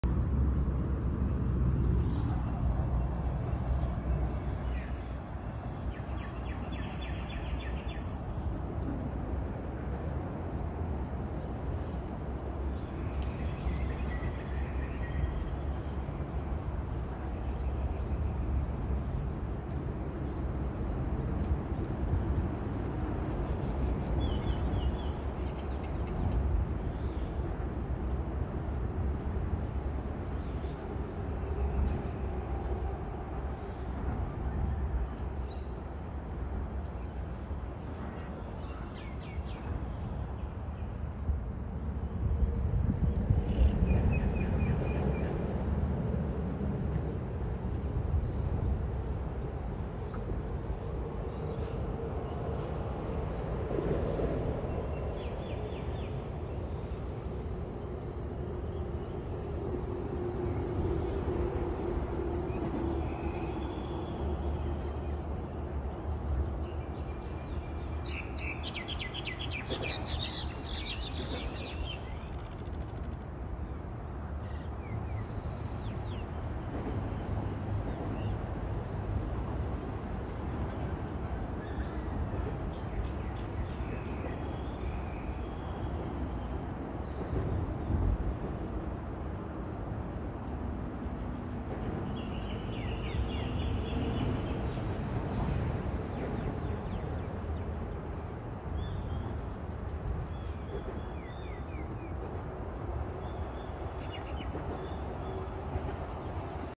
Life under the motorway
Duck River and life under the M4 motorway in Sydney